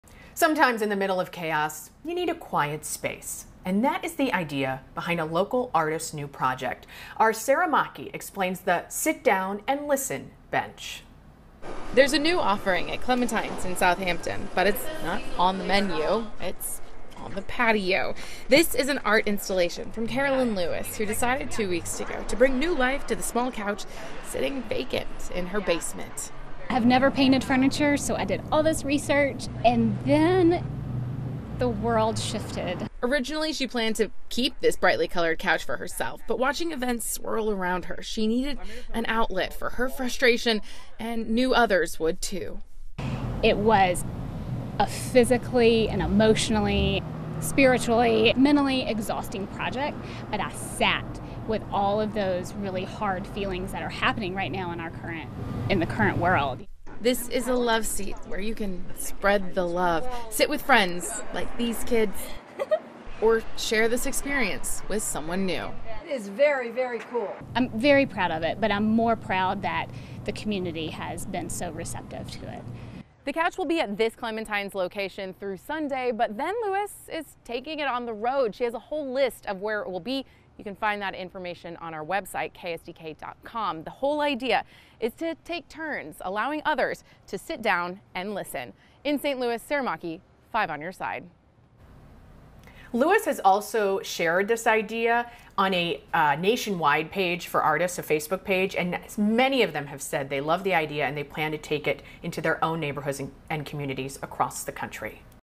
St. Louis art piece is designed to make you 'Sit Down & Listen'
ST. LOUIS — There's a new offering at Clementine's on Southampton, but it's not on the menu... it's on the patio.

Carolyn Lewis' art installation titled "Sit Down & Listen" occupies a corner of Macklind Avenue, inspired by the events of recent weeks.

"I have never painted furniture, so I did all this research to find out how to paint furniture that could be indoors and outdoors,” Lewis said of her initial plan to create something for her personal use. “But then the world shifted.”

A couple of weeks ago, she decided to pull the old couch that had been sitting unoccupied in her basement for the better part of a decade. Before that, it spent 22 years in her parents’ Alabama home.

Watching events unfold around her, she needed an outlet for her own frustration and knew others would, too.

“It was a physically, emotionally, spiritually, mentally exhausting project, but I sat with all of those really hard feelings that are happening right now in our country,” she said.

"Sit Down & Listen" is a love seat where you can spread the love, whether it be with old friends or a stranger. And already, Lewis said she sees people’s emotional reaction to it, saying some "just walk by and touch it, which is a beautiful thing to witness."

“I'm very proud of it," she added. “But I am more proud that the community has been so receptive to it.”

The couch will be at Clementine’s through Sunday, June 14, but she will move the installation around the St. Louis area, so others can sit down and listen.